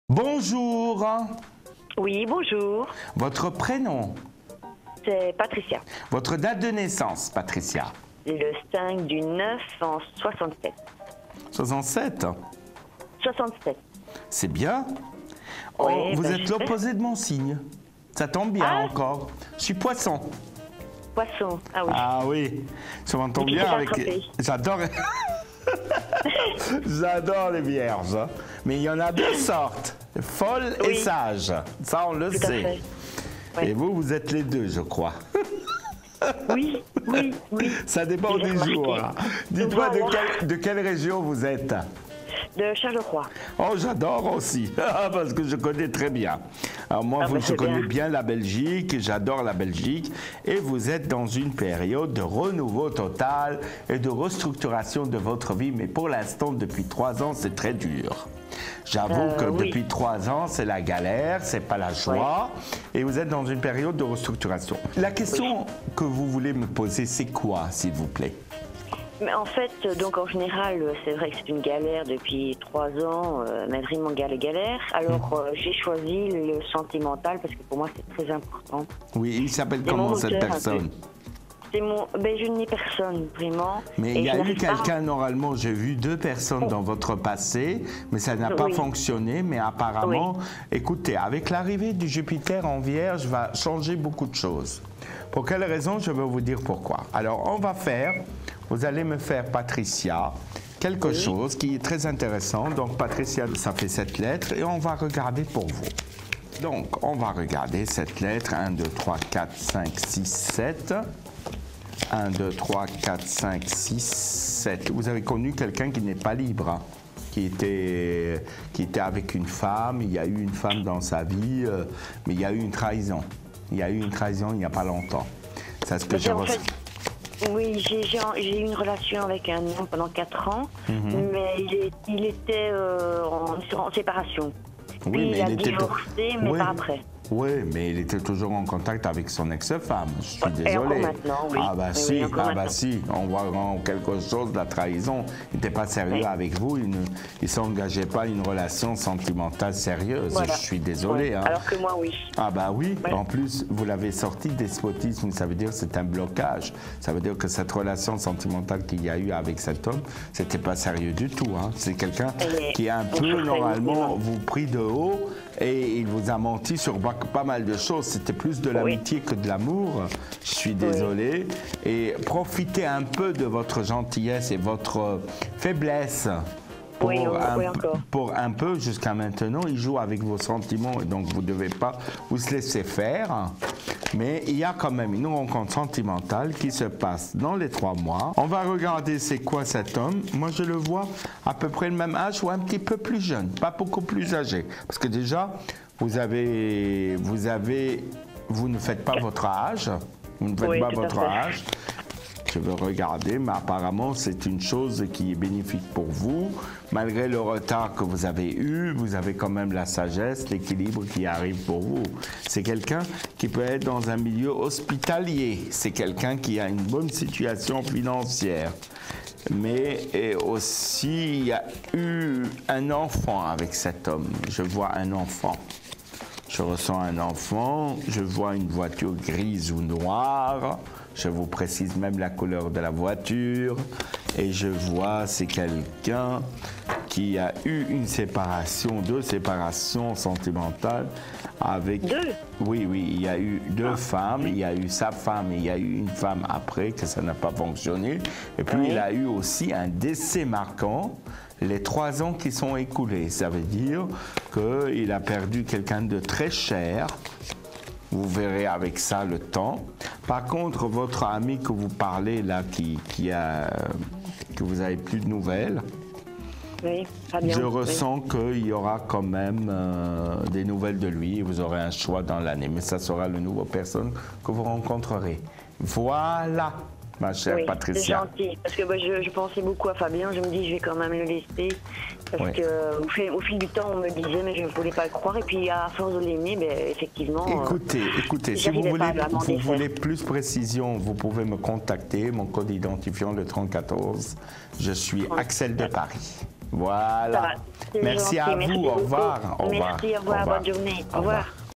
Quel est mon avenir sentimental ? 100% Voyance - Axel de Paris
Axel de Paris répond à vos questions.
Vous souhaitez faire une consultation avec Axel de Paris ? Rendez-vous sur sa fiche de présentation